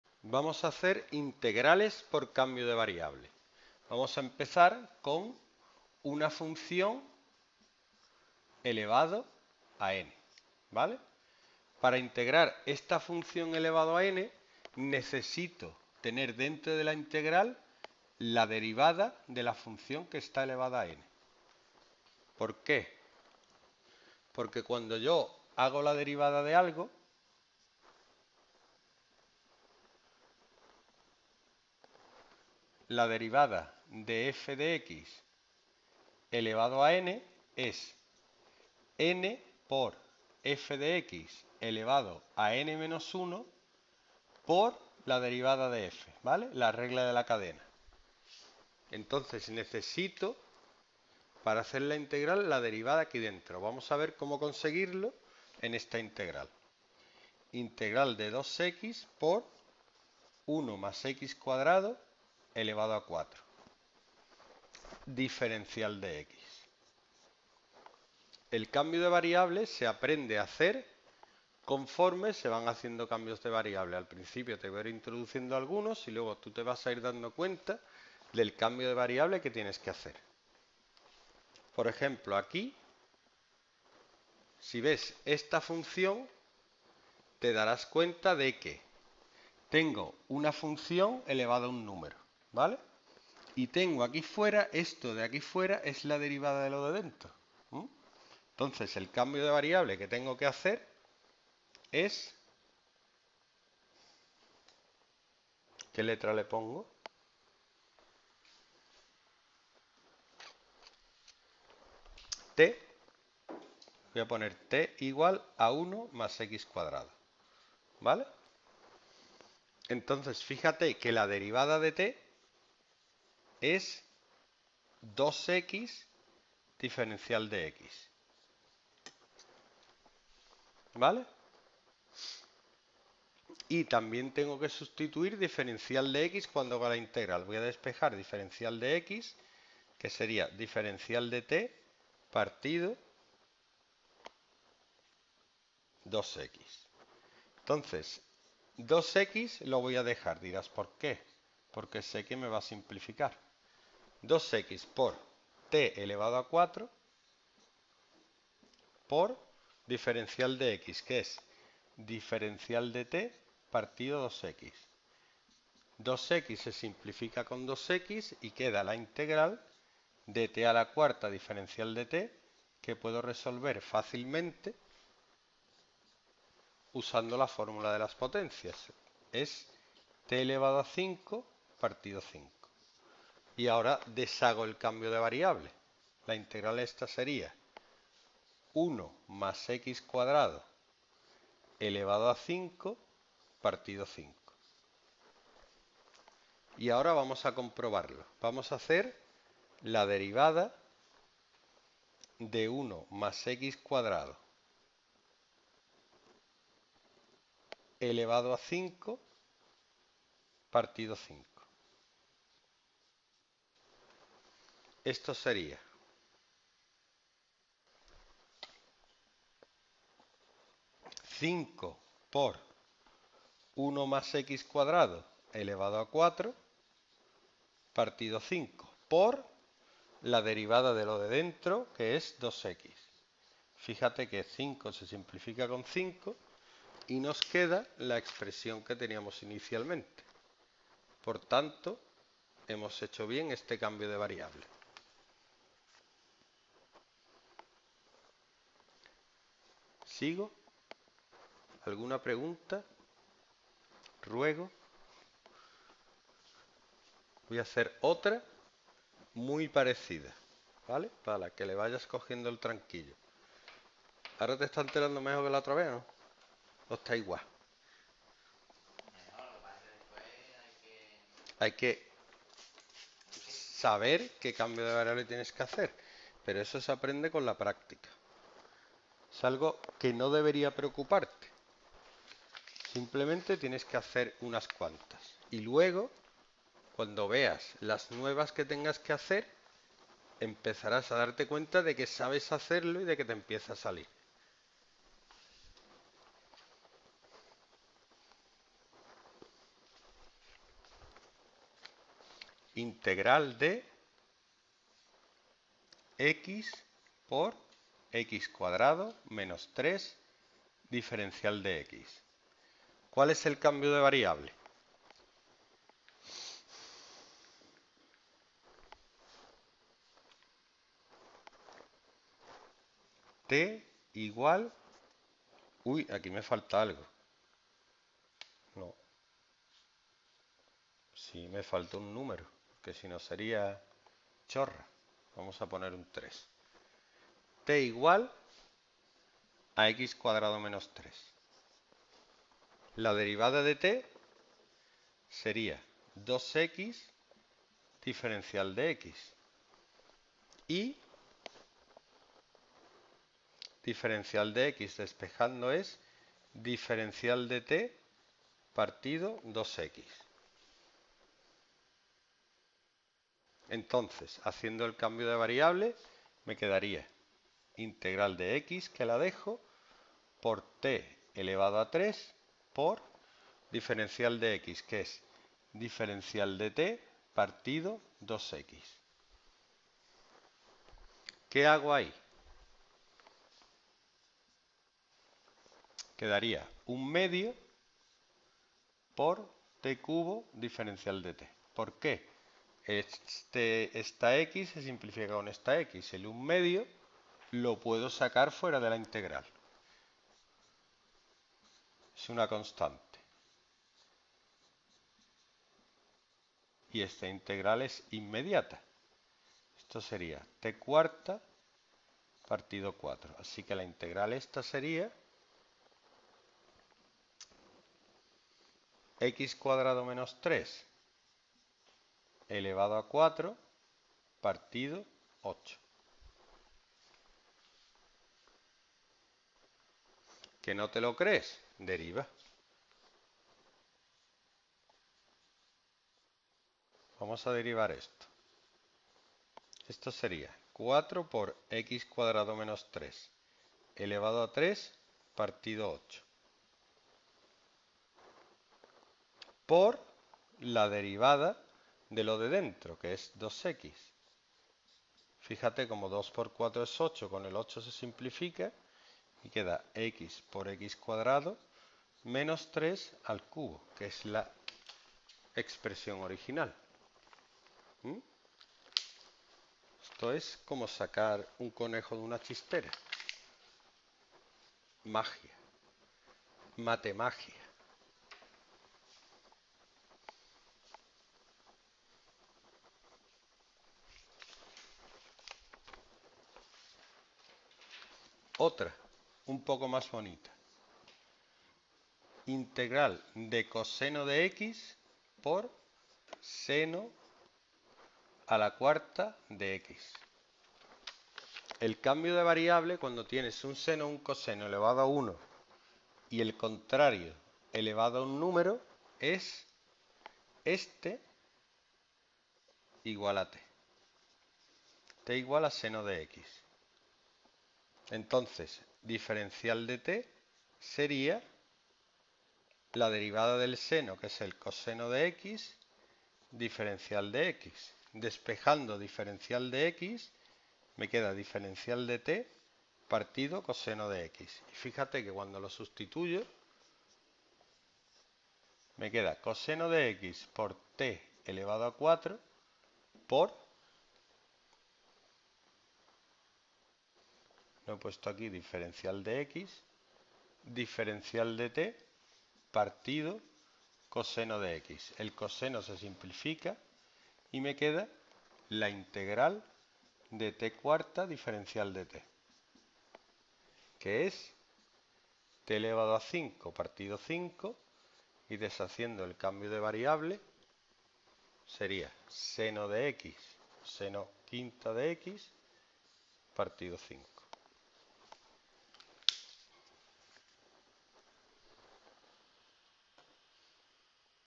Integral por sustitucion Funciones polinómicas Matemáticas 2º Bach Academia Usero Estepona
Integral por sustitucion Funciones polinomicas
Para realizar una integral por sustitucion o cambio de variable de una funcion polinómica hemos de hacer el cambio de variable polinomio=t y tenemos que tener la derivada del polinomio fuera de la expresión a integrar.
Academia Usero en Estepona, el centro de formación con más vídeos en Internet del Mundo presenta: Integral por sustitucion Funciones polinomicas